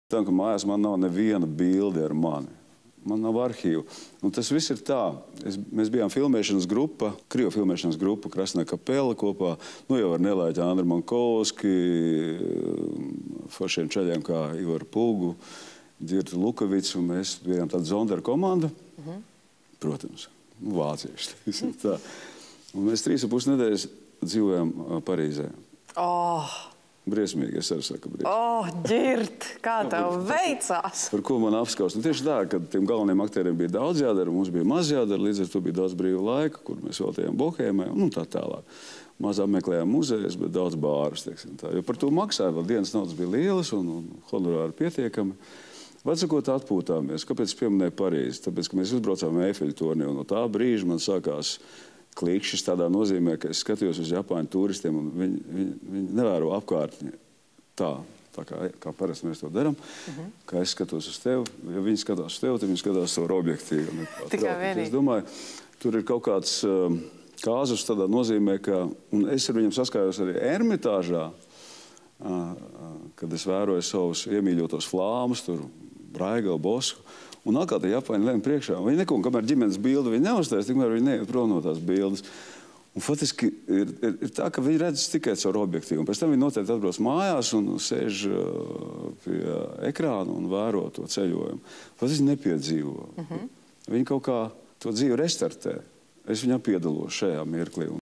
Ģirts Ķesteris: Citi dzīvi restartē - es tajā piedalos tagad un te
Ģirts Ķesteris: Cilvēki, kuri ceļojumu laikā uzņem daudz foto, dzīvo caur objektīvu. Atbraucot mājās, viņi šo "dzīvi" restartē. Es, savukārt, tajā piedalos - tagad un te.

Par dzīvi, bohēmu, ceļošanu, un, protams, mīlestību šovakar plkst.22:00 Viņas Ceturtdienas lielajā intervijā ar aktieri Ģirtu Ķesteri!